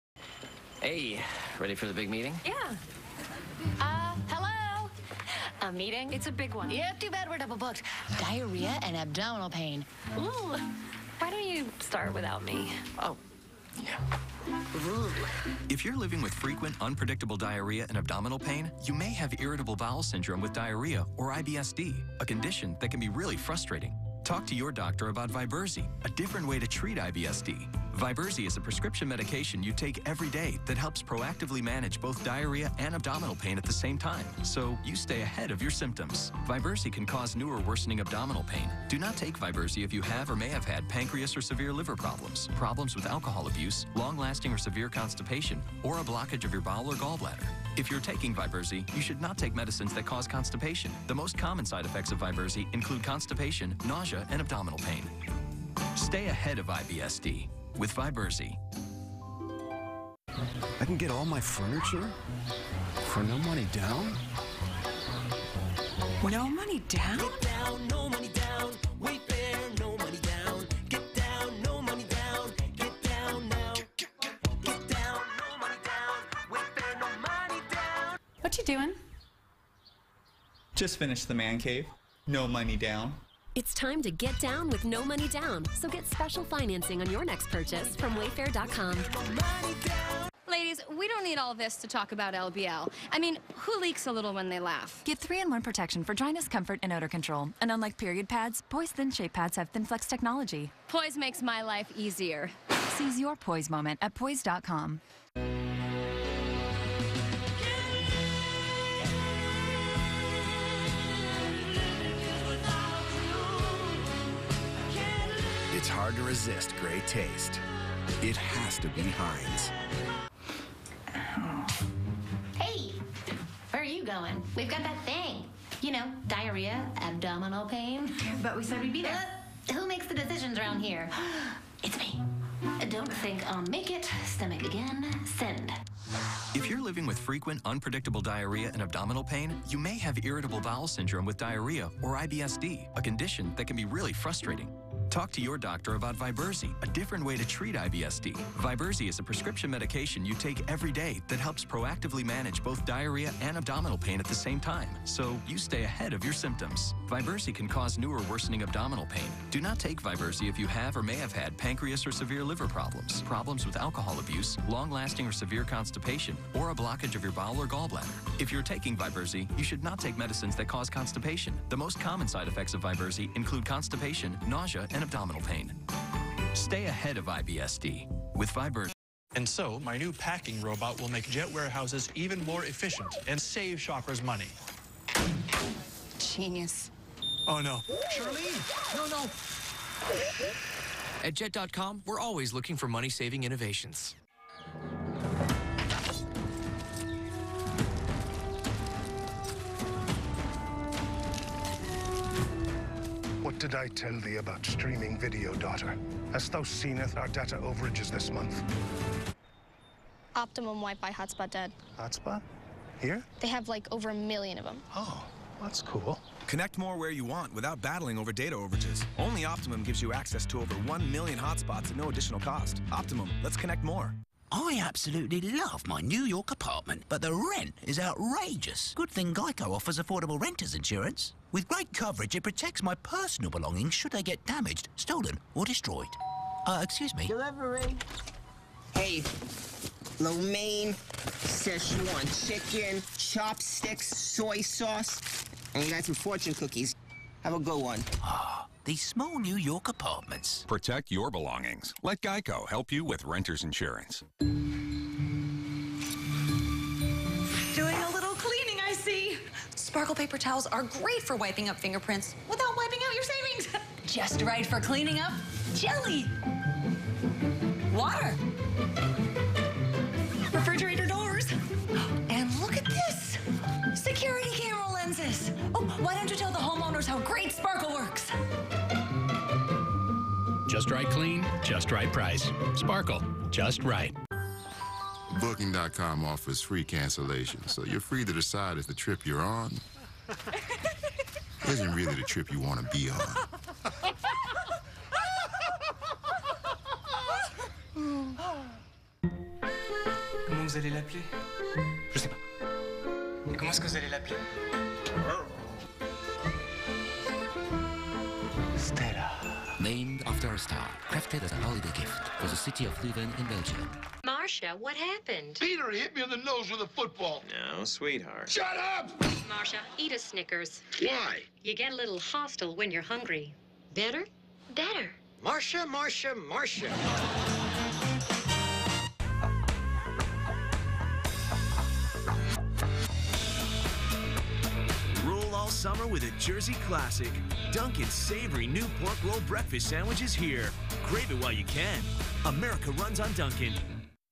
13 Very Odd TV Commercials that make you wonder What Were They Thinking? (2016)
These odd TV spots actually aired.  I'd love to be in the room during the creative pitch and final green light meeting :)

Viberzi - The Big Meeting starring Ilana Becker & Alison Becker
Wayfair - Getting Down for No Money Down
Poise Thin-Shape Pads - glamourous teaser starring Brooke Burke-Charvet
Heinz
Viberzi - Spend a day without abdominal pain starring Ilana Becker
Jet.com - Demise of a Packing Robot
Optimum - Getting medieval on streaming data!
Geico - protecting Gecko's Tiny New York Apartment
Sparkle - Just Right Fairy Security
Booking.com - Jordan & Chelsea's Wedding with Rebel Wilson, Keegan-                Michael Key
Stella Artois - Inspired by a Star
Snickers - Bizarro Brady Bunch starring Danny Trejo, Steve Buscemi
Dunkin - The New Pork Roll, a Jersey Classic